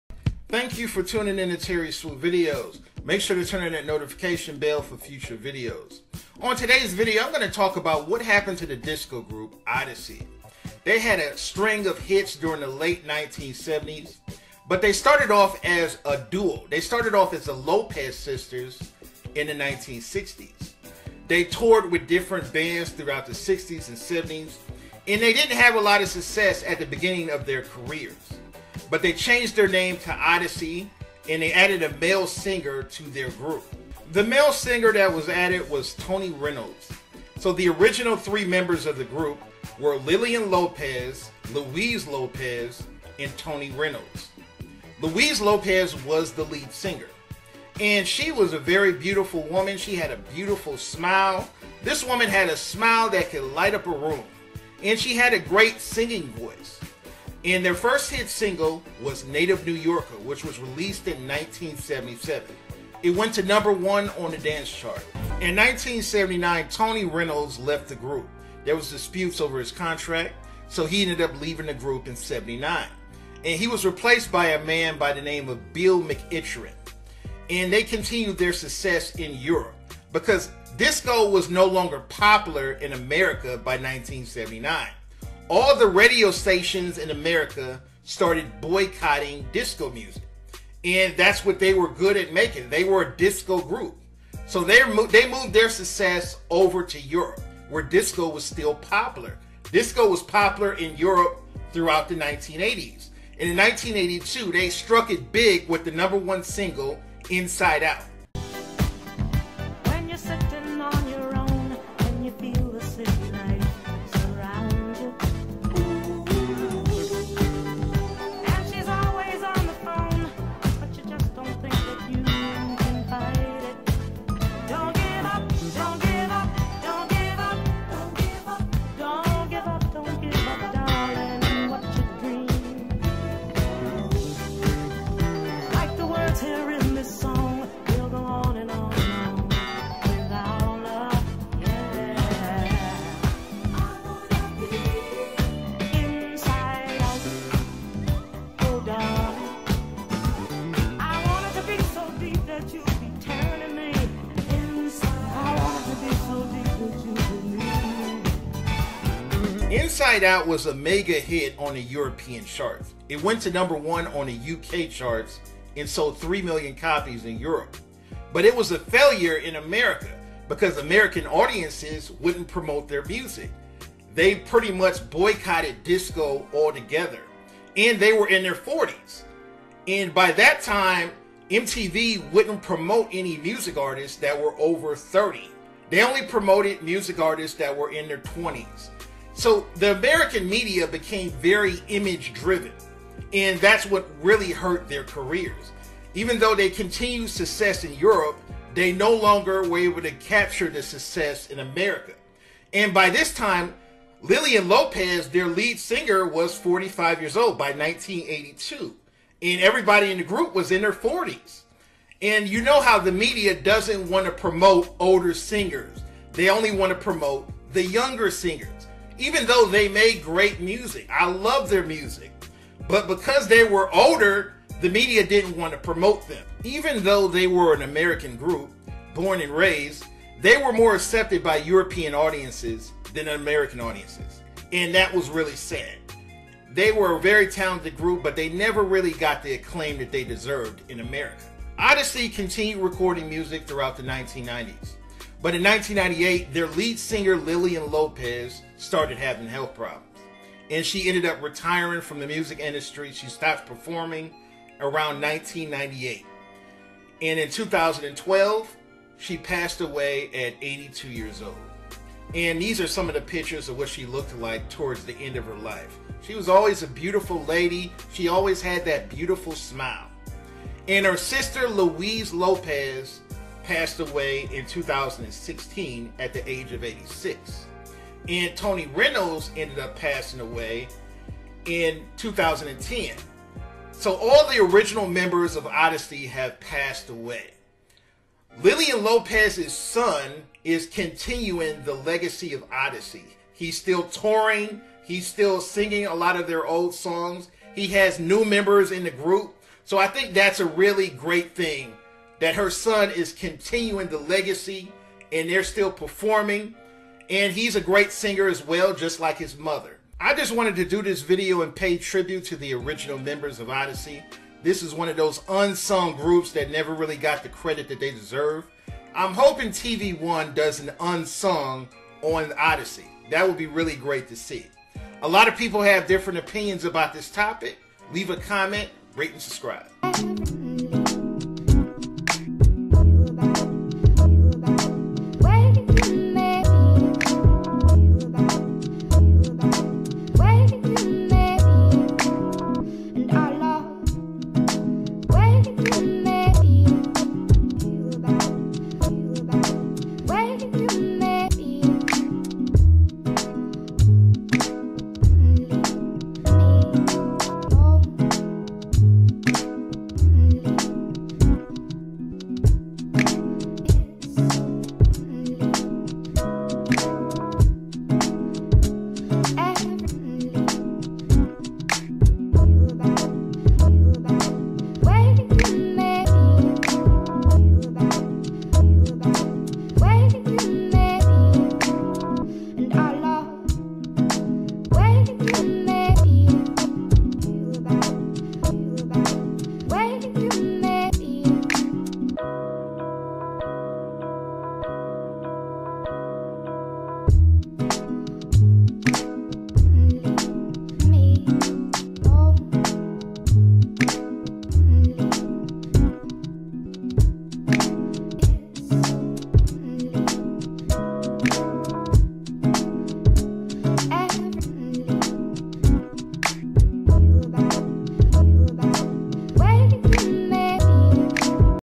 What Happened To Disco Group Odyssey
My commentary video on the 1970's disco group Odyssey.